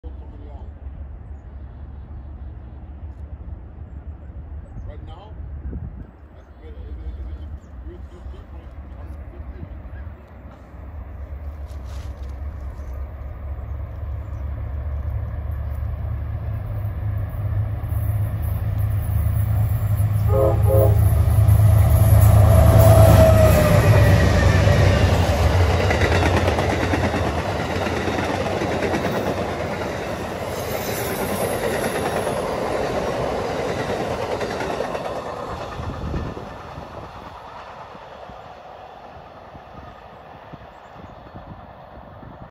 873: Amtrak 48 with friendly engineer
Led by p42dc 111 & 97 11/8/20